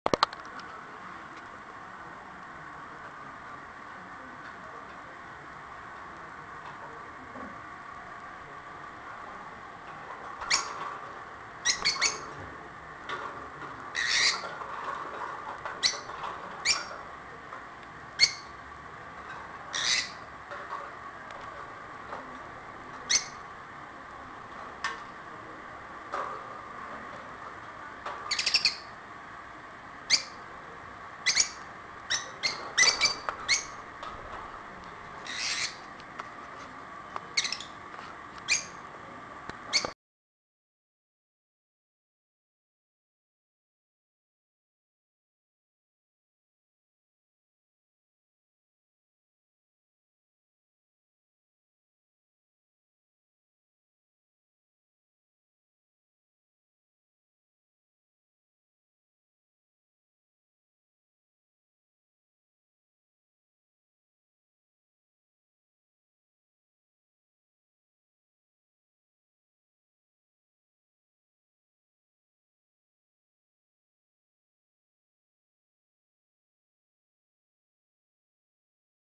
LoveBirds~1.mp4
My new pair of African Love Birds.